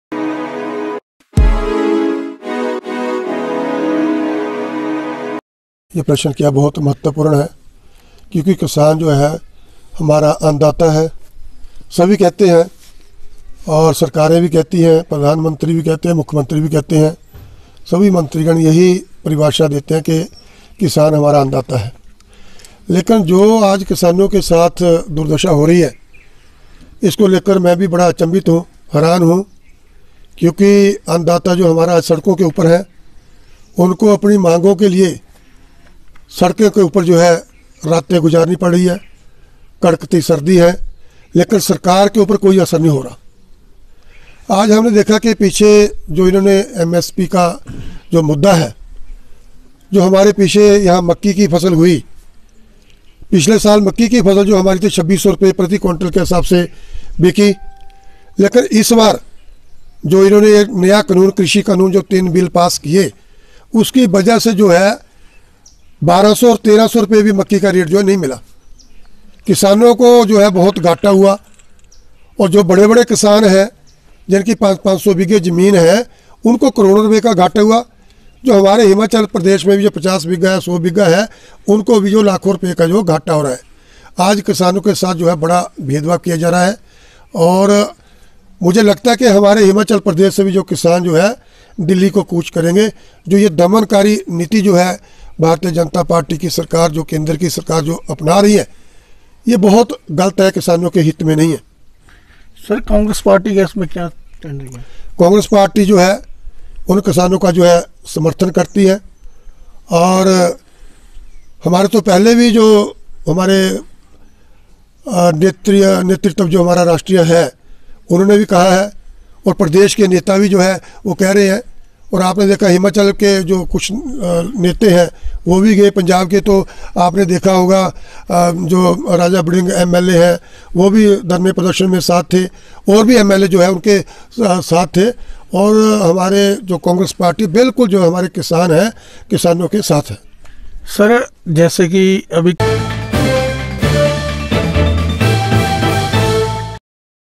सुने नालागढ़ विधानसभा क्षेत्र के विधायक लखविंदर राणा क्या कह रहे हैं किसानों के आंदोलन को लेकर
सुने नालागढ़ विधानसभा चुनाव क्षेत्र के विधायक लखविंदर राणा क्या कह रहे हैं किसानों के आंदोलन को लेकर